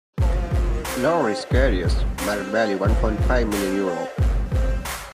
Loris Karius ⚽💥
Nick Pope: Suspended for three domestic games after red card against Liverpool. 🔴

Martin Dúbravka: Cannot play for Newcastle against Man Utd after featuring for the Red Devils in the EFL Cup twice. ❌

Newcastle's likely starter for the EFL Cup final against Man Utd: Loris Karius

Let's watch Loris Karius Stats...